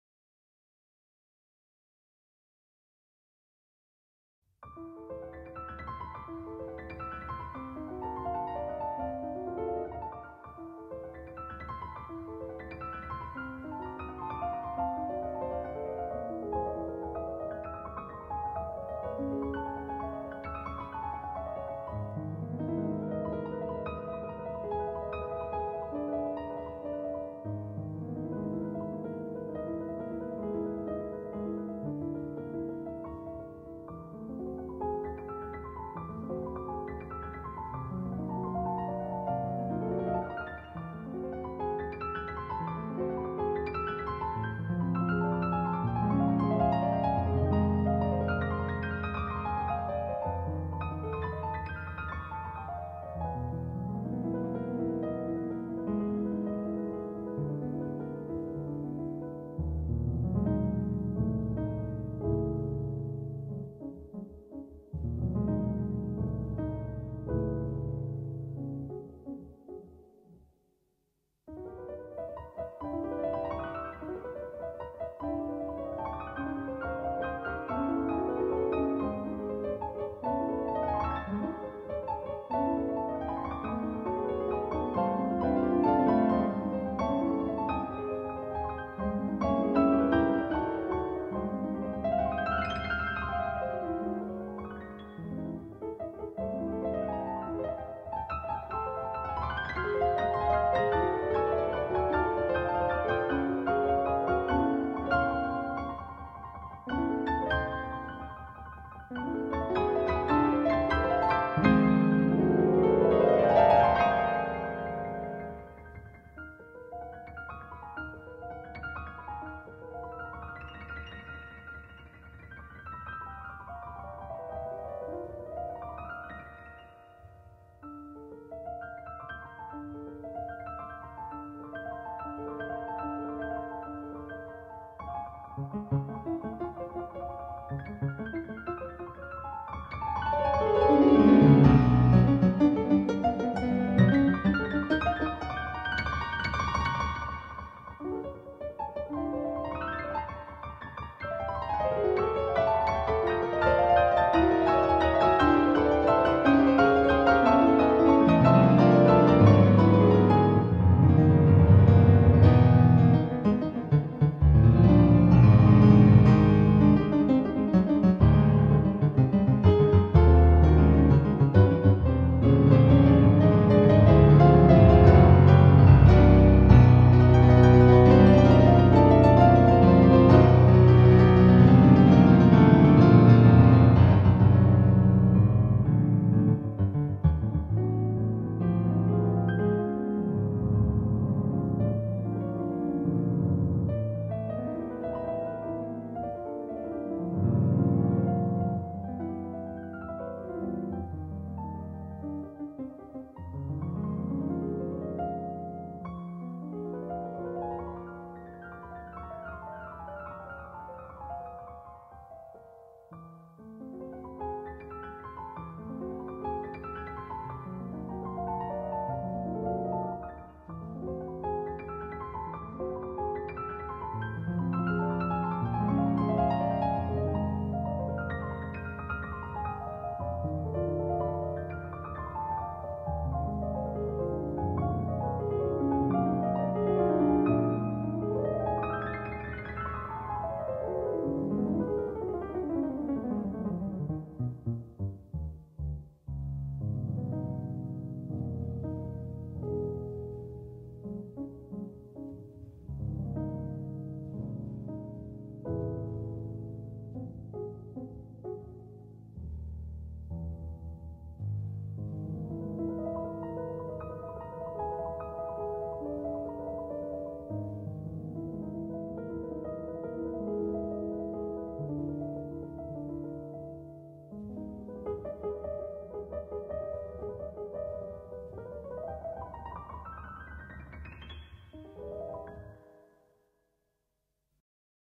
Microscopic Pond Life
Piano piece written and performed by William Moody